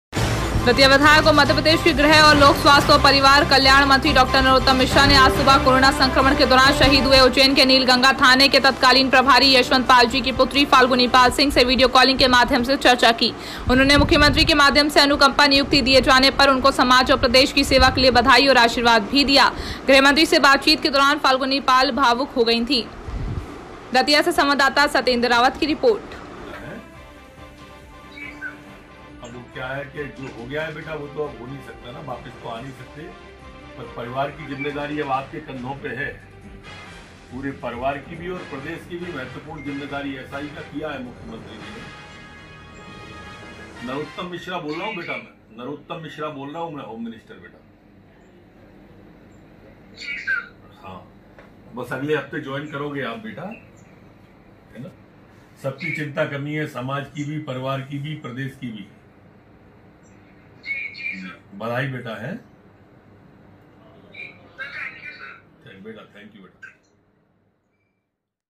फाल्गुनी पाल बनी सब इंस्पेक्टर दतिया विधायक एवं मध्यप्रदेश के गृह एवं लोक स्वास्थ्य एवं
स्लग-फाल्गुनी पाल बनी सब इंस्पेक्टर/दतिया/ संवाददाता/सत्येंद्र रावत/ दिनांक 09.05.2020

एंकर-दतिया विधायक एवं मध्यप्रदेश के गृह एवं लोक स्वास्थ्य एवं परिवार कल्याण  मंत्री डॉ नरोत्तम मिश्रा ने आज सुबह  कोरोना संक्रमण के दौरान शहीद हुए  उज्जैन के नीलगंगा थाने के तत्कालीन प्रभारी यशवंत पाल जी की पुत्री फाल्गुनी पाल सिंह से वीडियो कॉलिंग के माध्यम से चर्चा की । उन्होंने मुख्यमंत्री के माध्यम से अनुकंपा नियुक्ति दिए जाने पर उनको समाज और प्रदेश की सेवा के लिए बधाई और आशीर्वाद भी दिया। गृहमंत्री से बातचीत के दौरान फाल्गुनी पाल  भावुक हो गई थी।